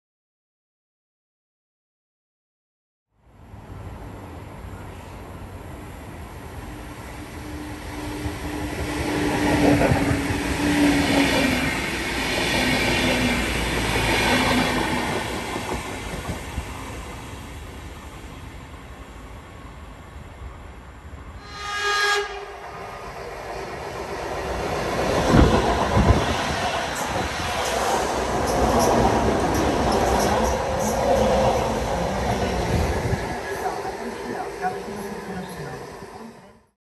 Renfe UT470 Regional Express (Barcelona/Estació de Bellvitge - 19/11/21)
Cruce de Regionales Express. Uno procedente de Madrid-Chamartin y otro con dirección Lleida-Pirineus.

00:04 Media Distancia Madrid Chamartin-Barcelona
00:22 Media Distancia Barcelona-Lleida Pirineus